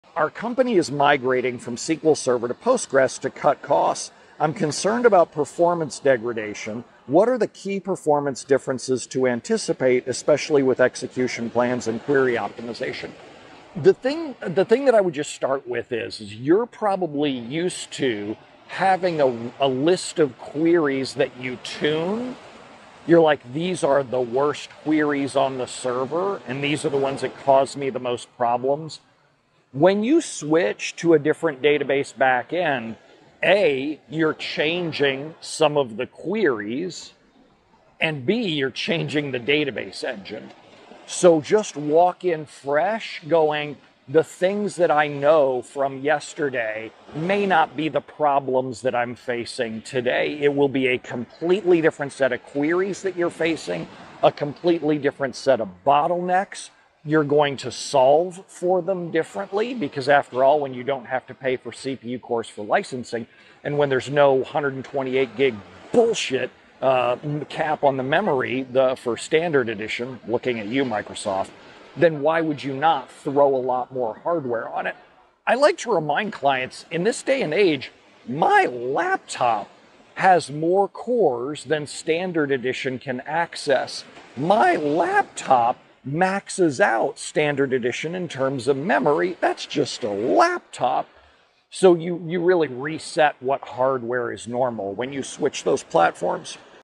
Migrating Platforms
11:07 JohnSteak: Our company is migrating from SQL Server to Postgres to cut costs. I'm concerned about performance degradation with our complex, ORM-generated queries. What are the key performance differences to anticipate, especially with execution plans and query optimization?